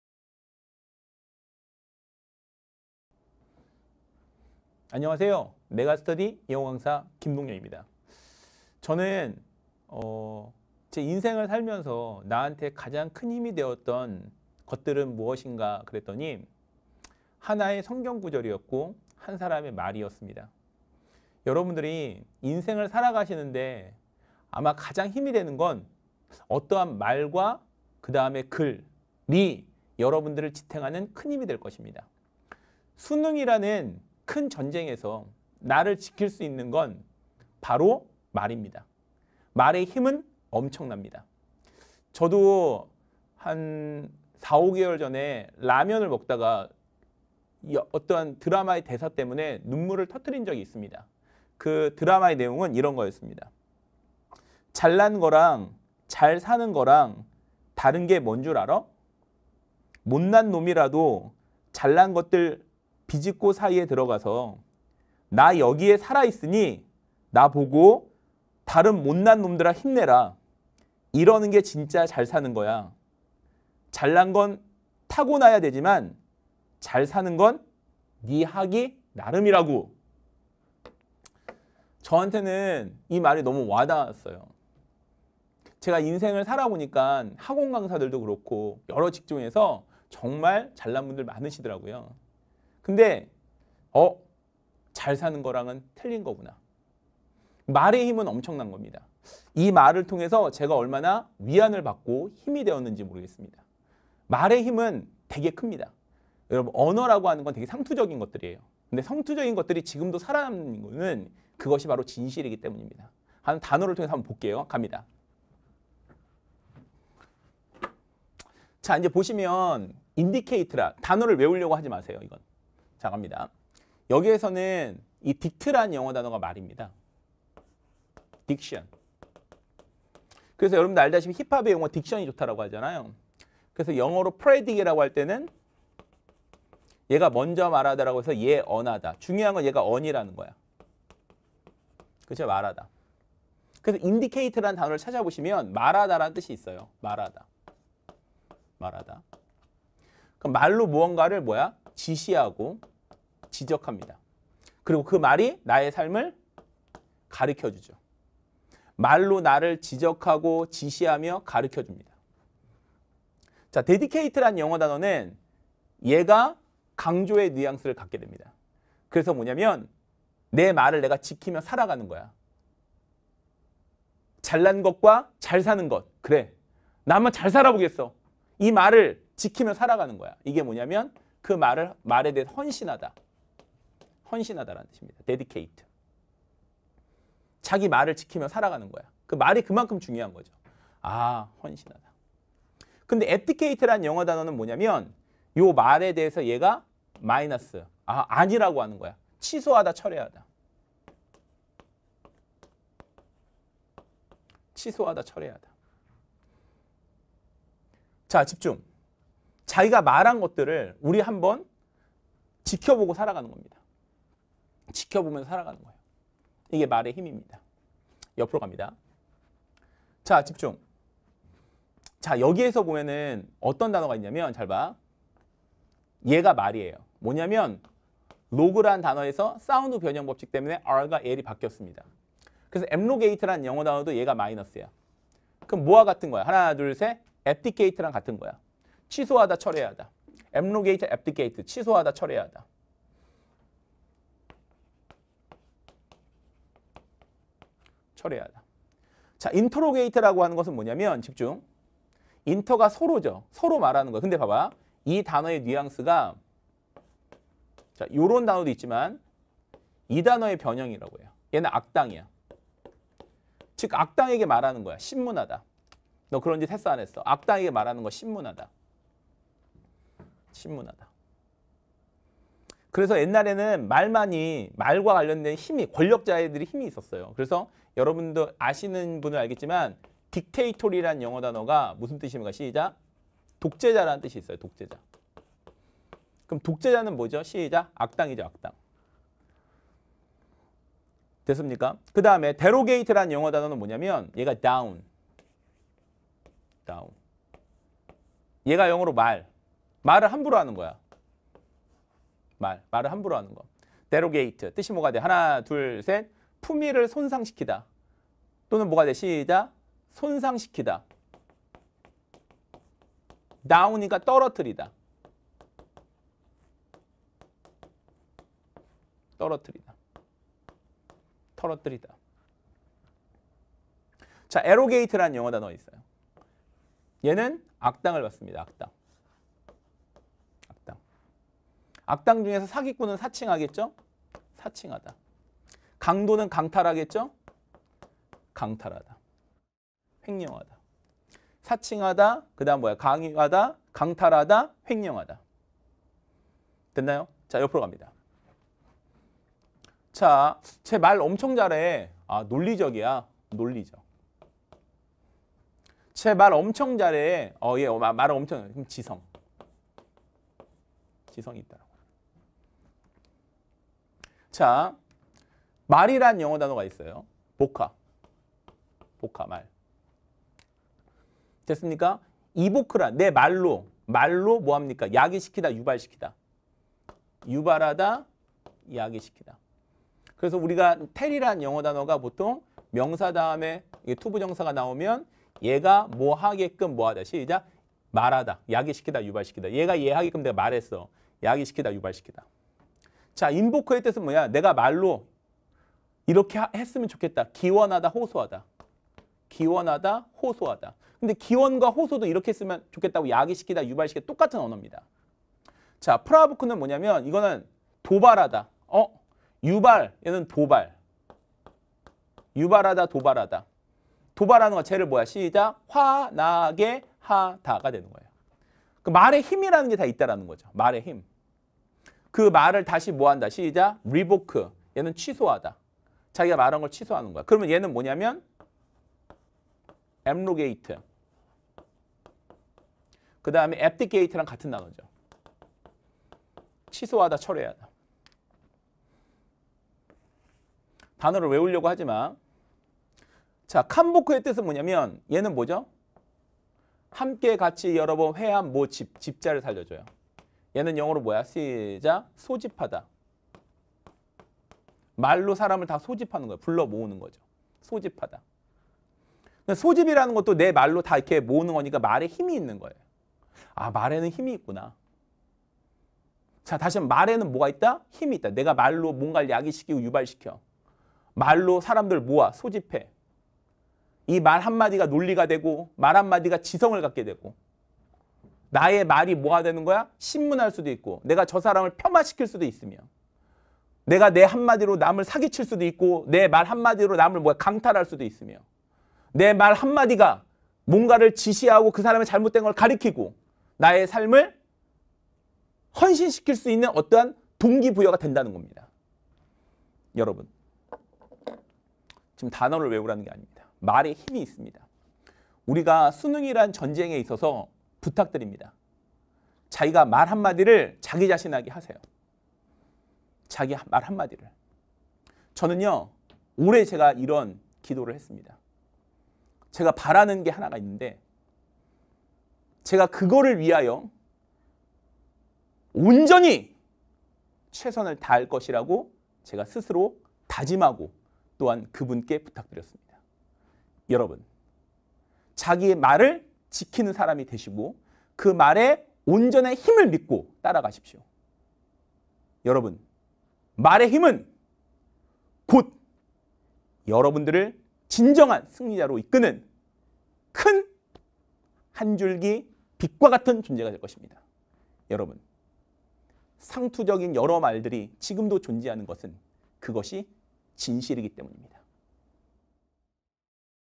[메가스터디] 영어 김동영 쌤 - 2021학년도 수험생에게 전하는 말의 힘☆Talk to Yourself!
2021학년도 수험생에게 전합니다!
Talk to Yourself!

여러분이 인생을 살아가면서 가장 큰 힘이 되었던 것은 '말'입니다.
수능이라는 큰 전쟁에서 나를 지킬 수 있는 것도 '말'입니다.

지금 김동영 선생님과 영단어를 통해
나에게 힘이 되어줄 만한 '말' 그리고 '단어'를 확인하세요!

▶김동영 선생님에 대해 더 알고싶다면?